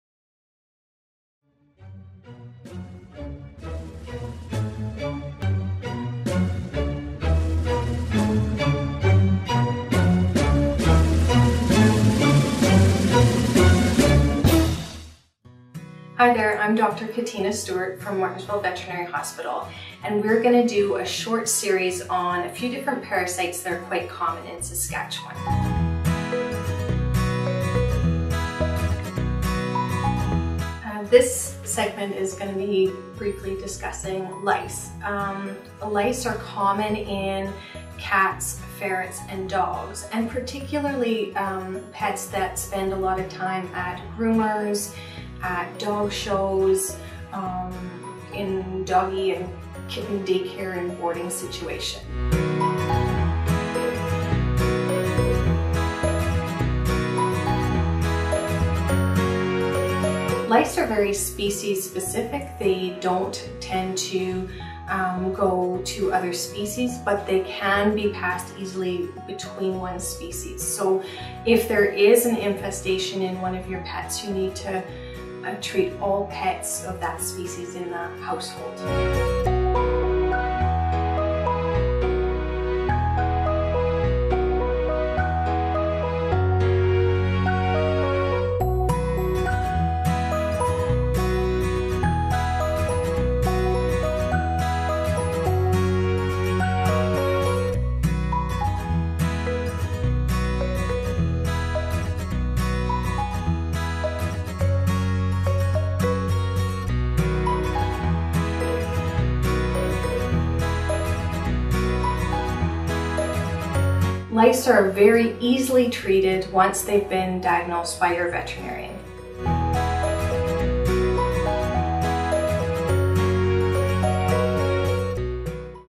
Parasites! Episode 2: Sucking Louse
This weeks Parasites! episode is on lice. Specifically, sucking lice. This is a common parasite in Saskatchewan that effects many different species, including cats and dogs.

If you have questions or comments we'd love to hear them!